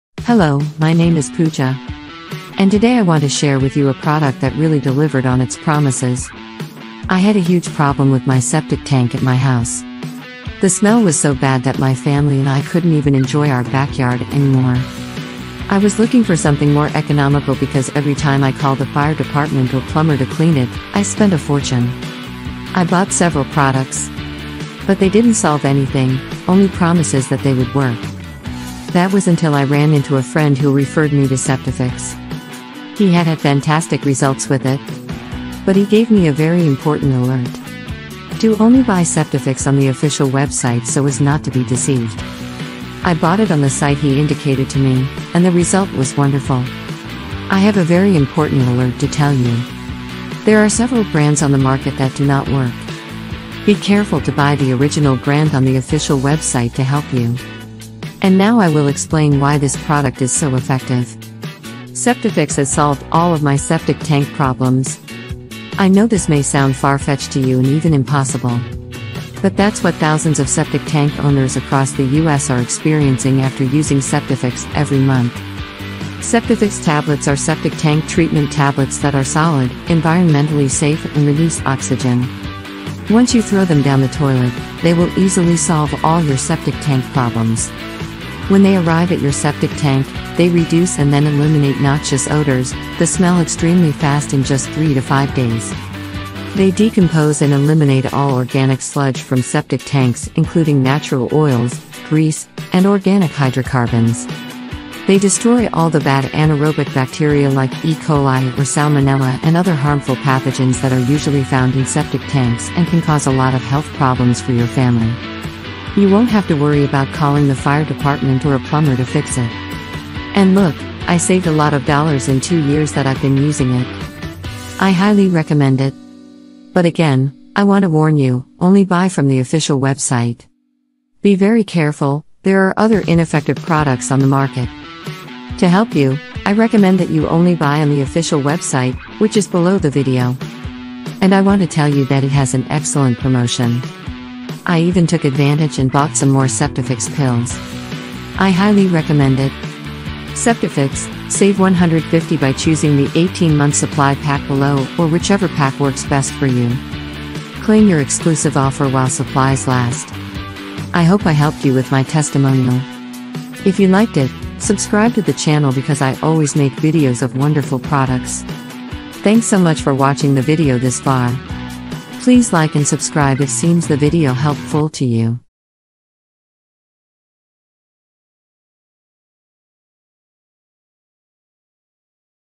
SEPTIFIX REVIEW 2023 || SEPTIC TANK TREATMENTS With Septifix
Septifix has solved all of my septic tank problems.

I know this may sound far-fetched to you and even impossible,

but that's what thousands of septic tank owners across the U.S are experiencing after using Septifix every month.

Septifix tablets are septic tank treatment tablets that are solid, environmentally safe and release oxygen.

Once you throw them down the toilet, they will easily solve all your septic tank problems.

When they arrive at your septic tank, they reduce and then eliminate noxious odors, the smell extremely fast in just three to five days.

They decompose and eliminate all organic sludge from septic tanks including natural oils, grease, and organic hydrocarbons.

They destroy all the bad anaerobic bacteria like E. coli or salmonella and other harmful pathogens that are usually found in septic tanks and can cause a lot of health problems for your family.